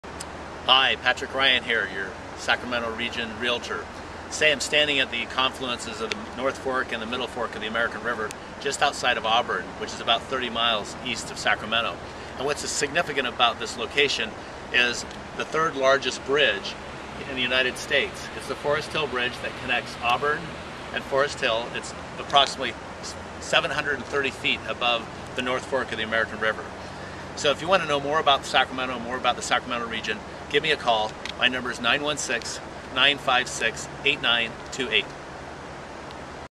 The Foresthill bridge was built to better access the Auburn and Foresthill communities as the plan was to dam the North and Middle Fork of the America river, creating a lake which would cover he roads used at the time to handle the traffic...  The bridge is the 3rd highest in the US at 730 feet!